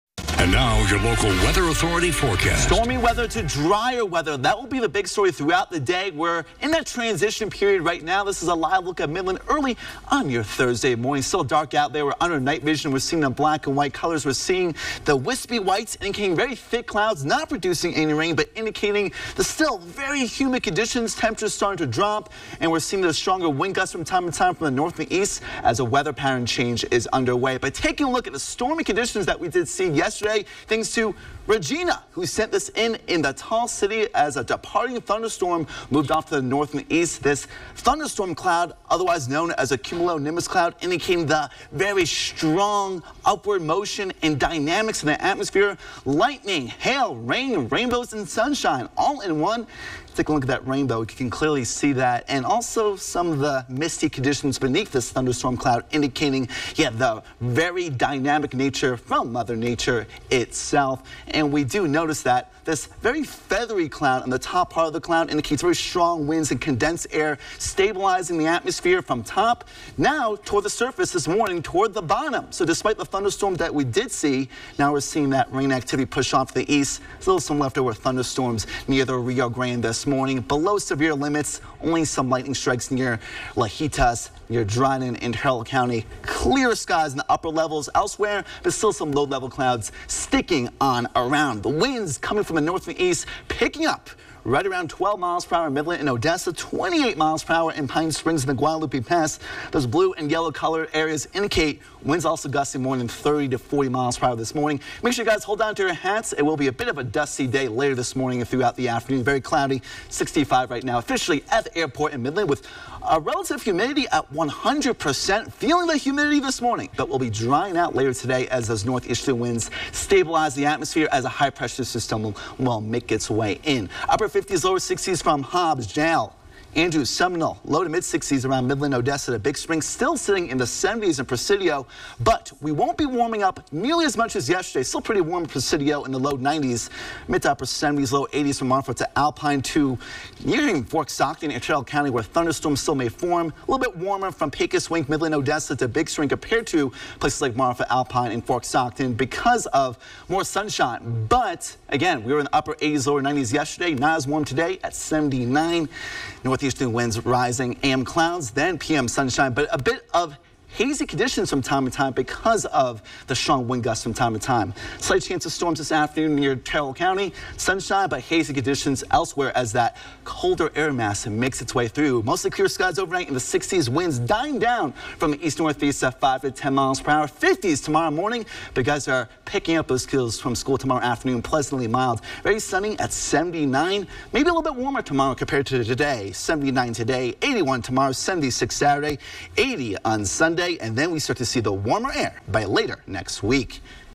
Fewer Basin Clouds & Some 70’s 10-5-2023
A more-stagnant air mass has overtaken The Basin as despite some morning thunderstorms, KMID / KPEJ Weekday Morning Meteorologist Ryan DePhillips highlights how most will become free from rain. Seasonable temperatures will eventually reach the high 60’s, 70’s, and lower 80’s this afternoon as persistent winds will generally come from the east-northeast with patchy dust. Any thunderstorms that do form will be in far-southeastern areas and in far-southwestern areas of The Trans Pecos. Humidity will be lower Basinwide as a high pressure system to the northeast strengthens. Temperatures will remain slightly below normal for the next few days.